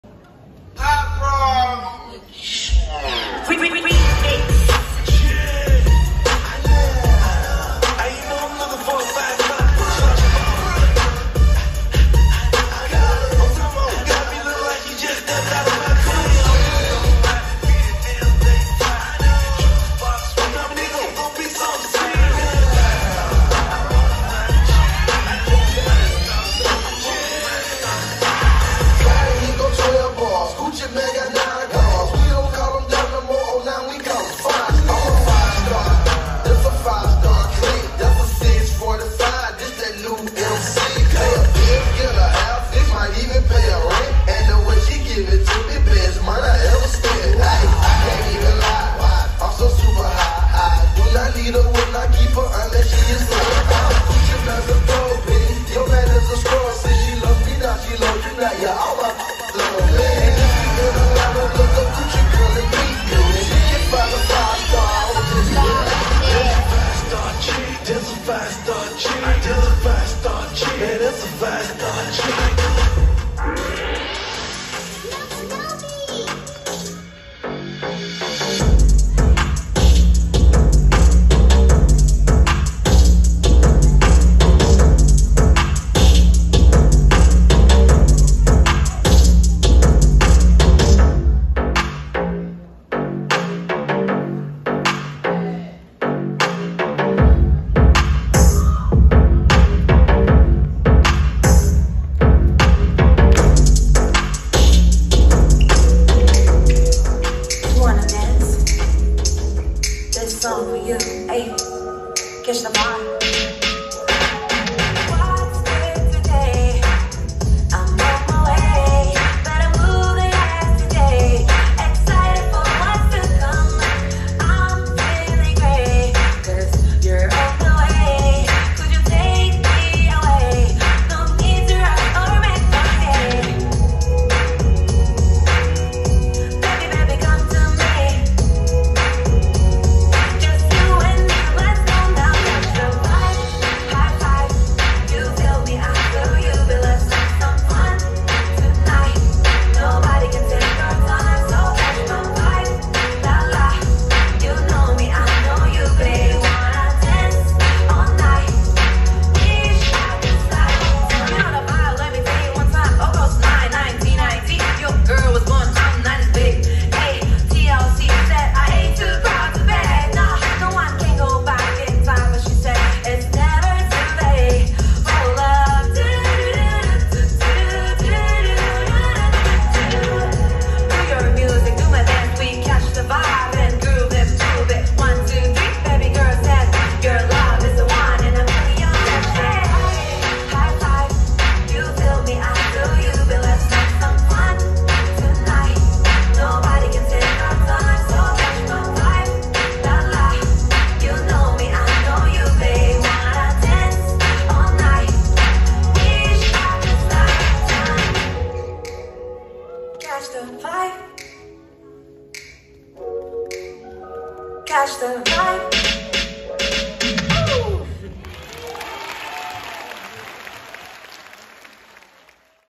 ⑫筑後HIPHOP入門 りゅう・さあやクラス【発表会2024】
発表会2024の様子です、今回は皆さんから共有して頂いた動画を投稿しております。
ありがとうございましたm(__)m

主に「ダンス」や「踊ってみた」「レクチャー」動画をUPしています
是非一緒に踊りましょう♫